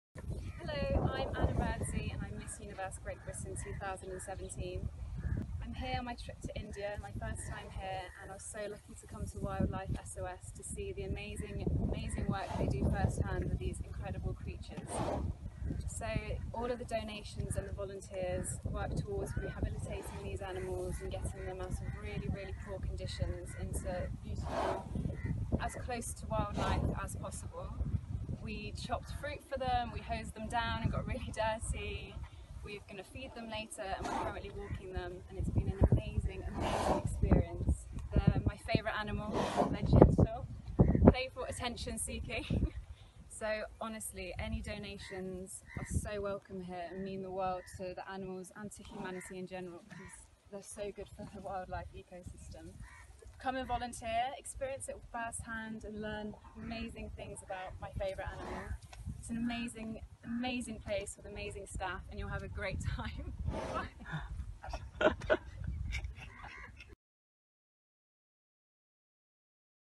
Miss Universe Great Britain 2017 Anna Burdzy Volunteers At Wildlife SOS!
Wildlife SOS was extremely happy to play host to Miss Universe Great Britain 2017, Anna Burdzy as she and her team spent two days volunteering at our Elephant Conservation And Care Center and Agra Bear Rescue Facility in Uttar Pradesh to learn and spread awareness about our work in conservation. Here's what she has to say about her experience..